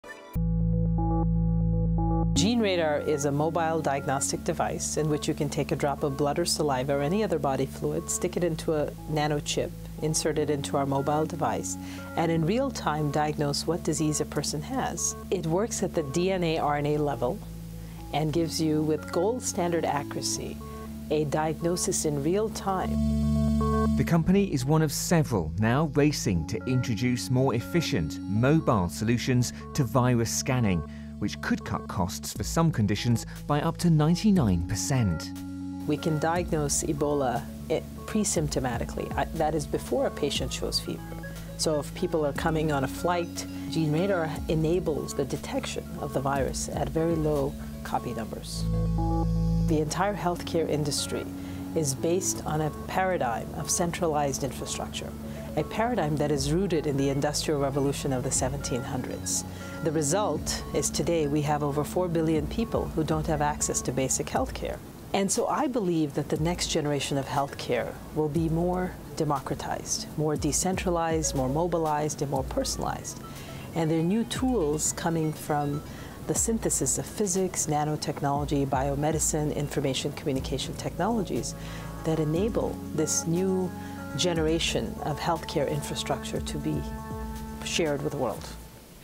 Mobile scanner detects disease from a drop of blood
A mobile scanner that can detect whether a person has Ebola, HIV or the flu virus in less than one hour has been developed.
Nanobiosym's Gene Radar decodes DNA from a drop of blood to diagnose conditions. Nanobiosym is one of several companies, including Corgenix and Nanomix, aiming to create an "instant" test for Ebola and other viruses or diseases that could eventually replace hospital tests.The company is waiting for approval from the US Food and Drug Administration (FDA) before offering the device for sale.